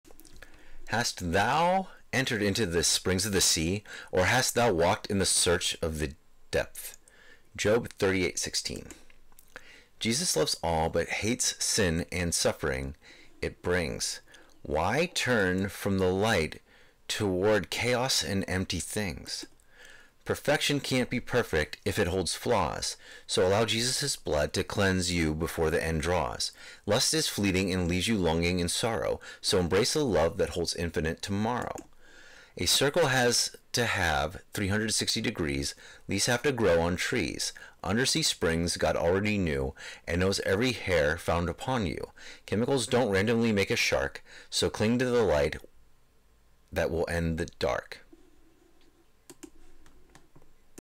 A Christian poem job38 16p15
A Christian poem from "First Fruits" written and read by lddj